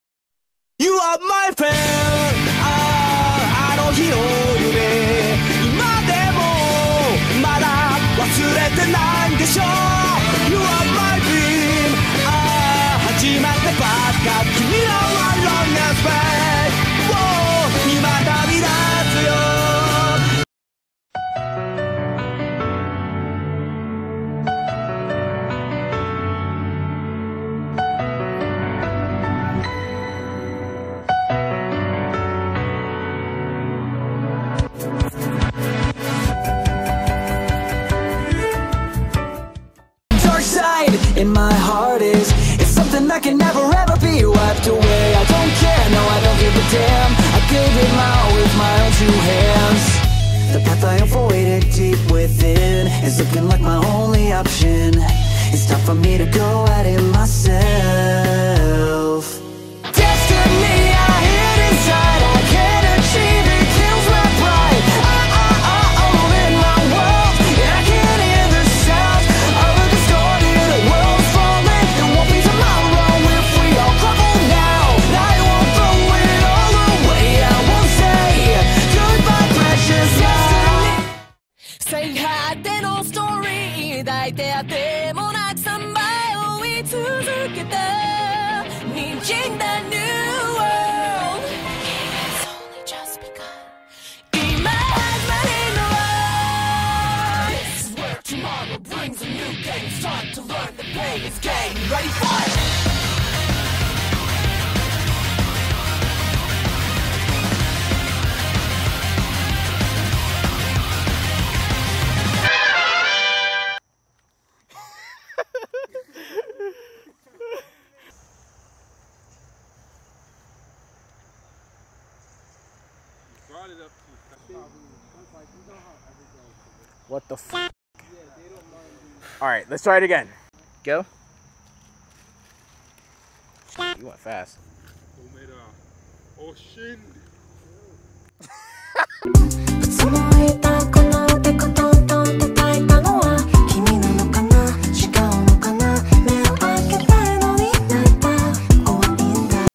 REAL LIFE ANIME OPENINGS!!! (On a Budget)
What if anime is in real life... but on a budget!
Can you guess the anime opening? Totally a meme